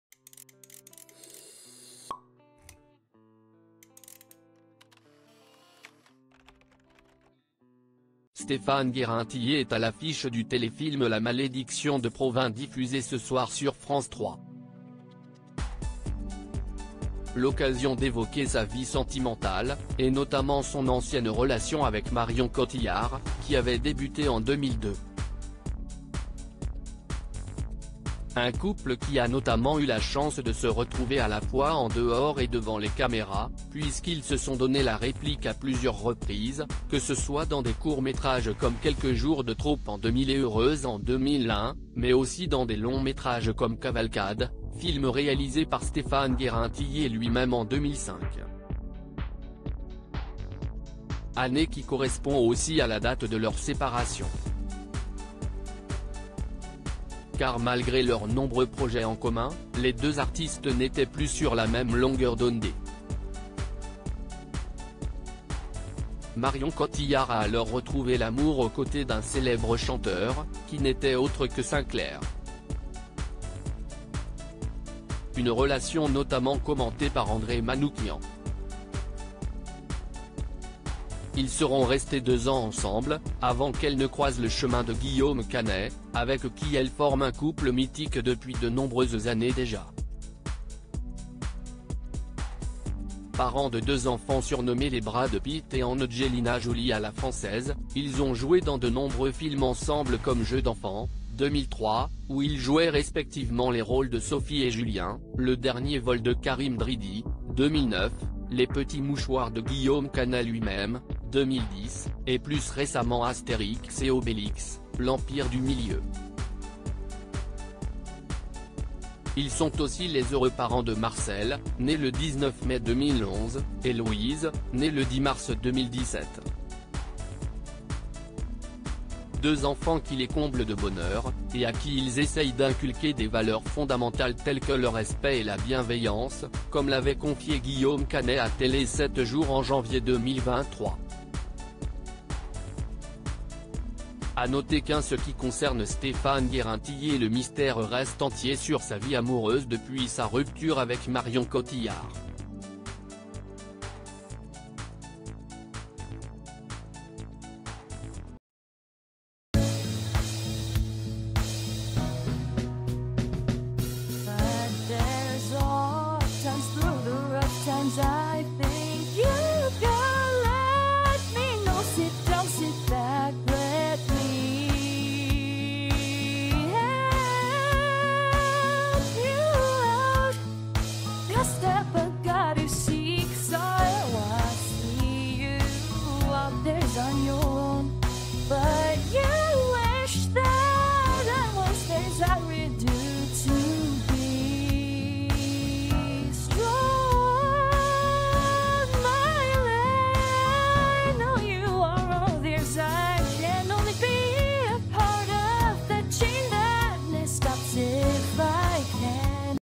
Stéphan Guérin-Tillié séparé de Marion Cotillard : l'actrice a craqué pour un célèbre chanteur après
Stéphan Guérin-Tillié séparé de Marion Cotillard : l'actrice a craqué pour un célèbre chanteur après leur rupture